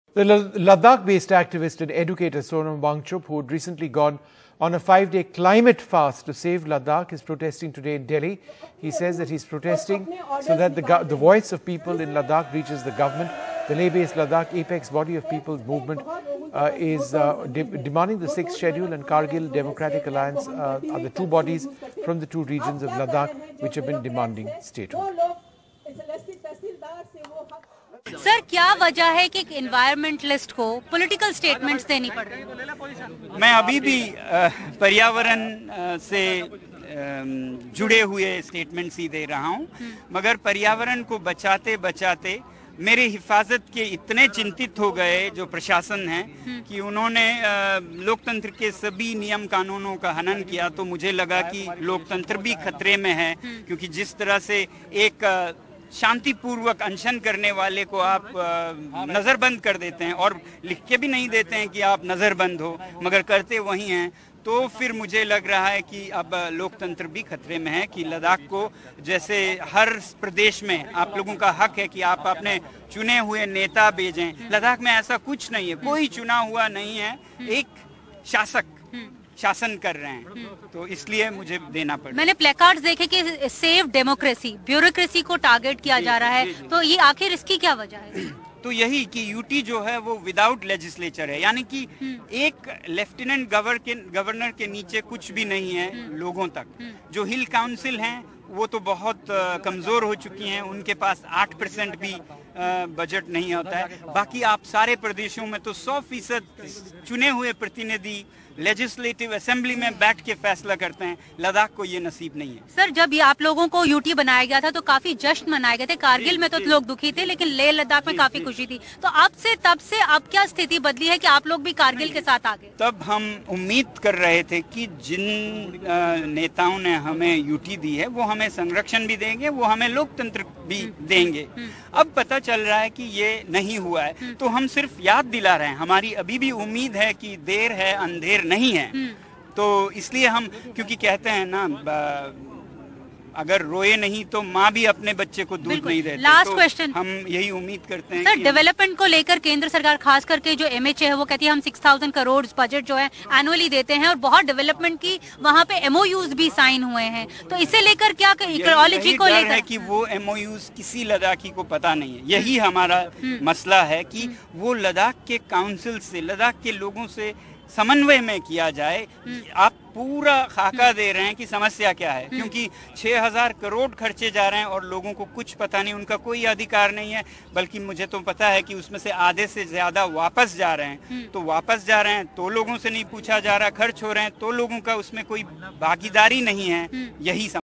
Ladakh Innovator Holds Protest in Delhi Seeking Statehood
Ladakh-based Activist and Educator Sonam Wangchuk, Who Recently Went on a Five-day “climate Fast” to “save Ladakh”, Held a Protest in Delhi Today. He Said the Voices of People in Ladakh Must Reach the Government. The Leh-based Ladakh Apex Body of People's Movement for 6th Schedule and Kargil Democratic Alliance From the Two Regions of Ladakh Have Been Demanding Statehood.